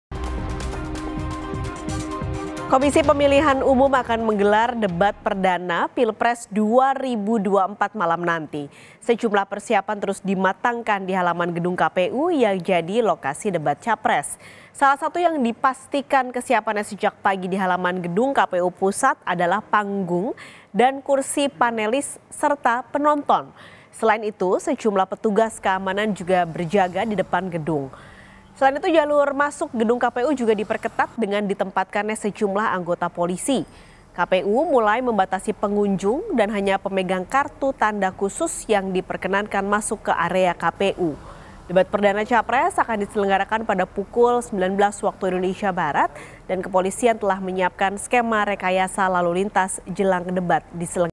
Jalan Depan Gedung KPU Ditutup Karena Ada Debat Perdana Capres!
KOMPAS.TV - Pada malam ini (12/12), Komisi Pemilihan Umum (KPU) akan menggelar debat perdana Pilpres 2024 di Gedung KPU. Persiapan telah matang sejak pagi di Halaman Gedung KPU Pusat.

Panggung debat, kursi untuk panelis, dan penonton telah dipersiapkan sejak pagi. Di depan gedung, sejumlah petugas keamanan bersiaga. Anggota polisi juga memperketat jalur masuk ke Gedung KPU.

KPU menerapkan pembatasan pengunjung, hanya pemegang kartu tanda khusus yang diizinkan memasuki area KPU. Kepolisian telah menyiapkan skema rekayasa lalu lintas untuk mengantisipasi keramaian sebelum debat.

Debat perdana Capres dijadwalkan dimulai pukul 19.00 WIB. Antusiasme masyarakat terhadap acara ini tampak tinggi, dengan langkah-langkah keamanan yang ketat untuk memastikan kelancaran jalannya debat.